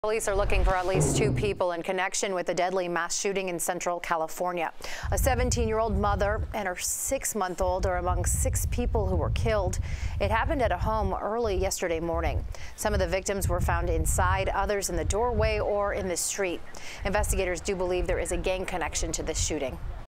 6 Killed In CA Mass Shooting
A teen mom, her 6-month-old, and four others were among those killed during a mass shooting in California.